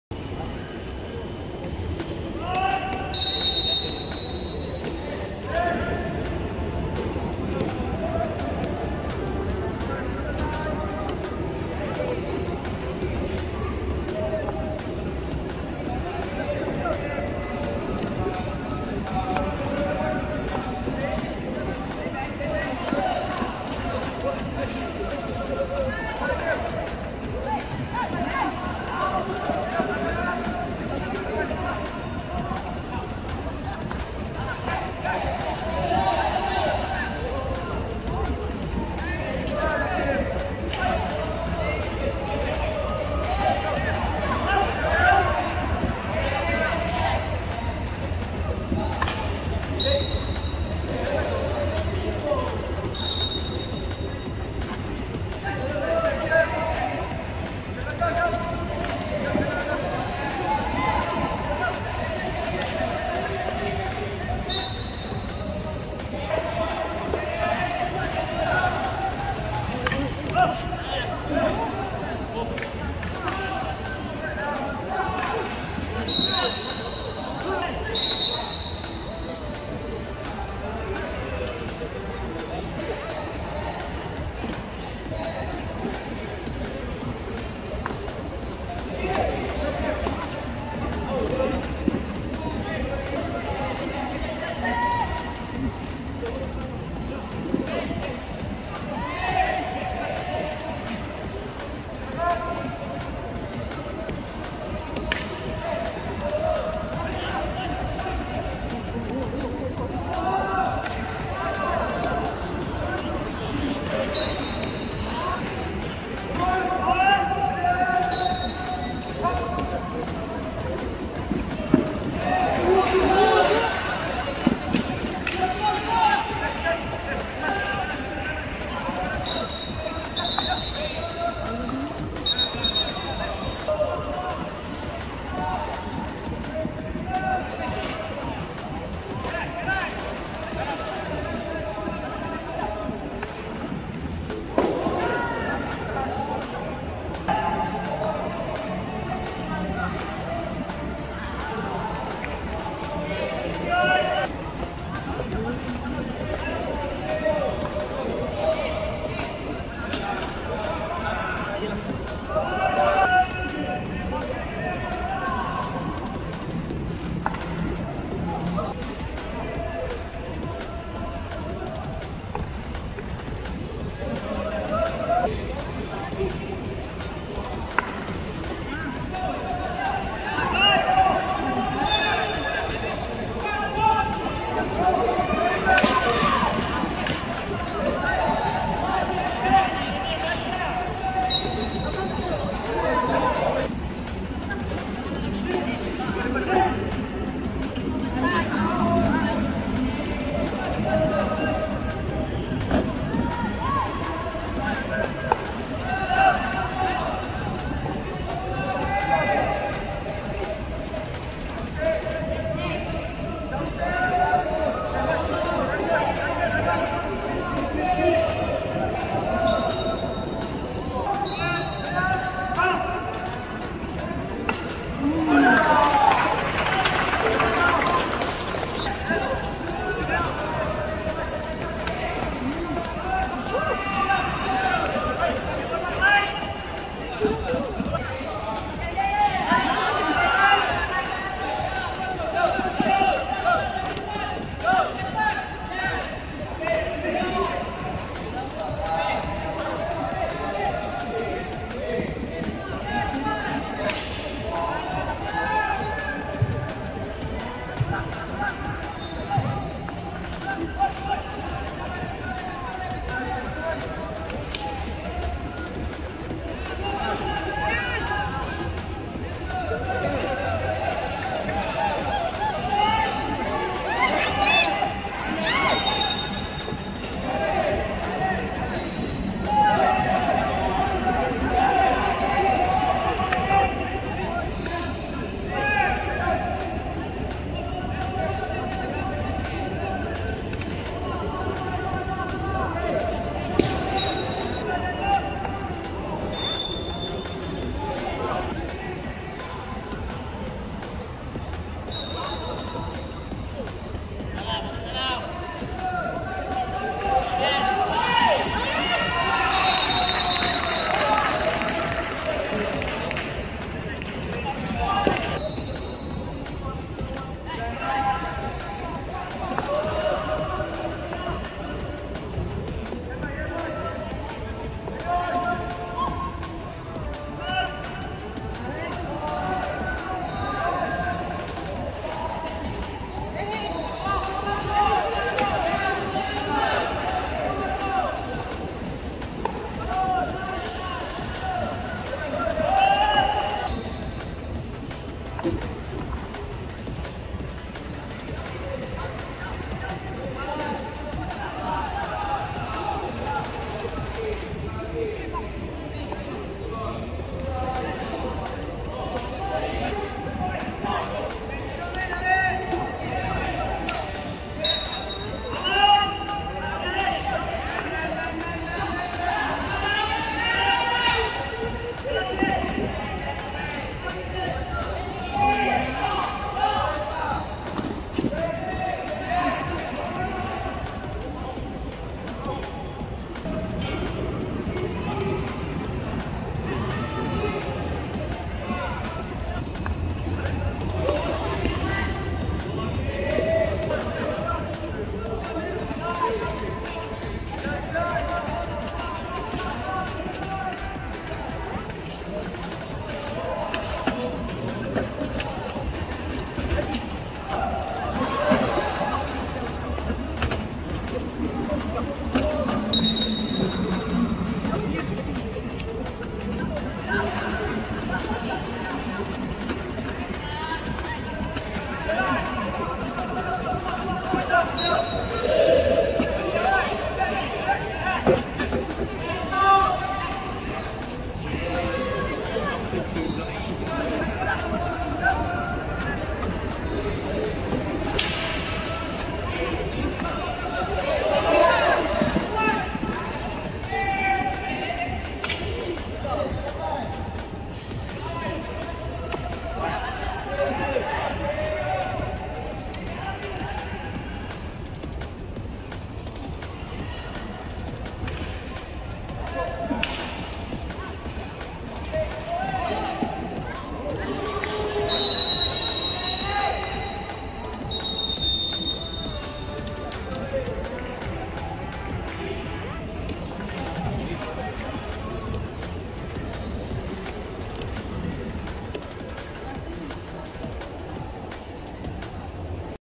NHDP Zon Tgh - Bangsar vs Putrajaya 1st Half
PLD Bangsar vs PLD Putrajaya.
Kejohanan Hoki NHDP Zon Tengah Bwh 17 Lelaki 2024, 14-16 Ogos 2024, Stadium Hoki Bukit Jalil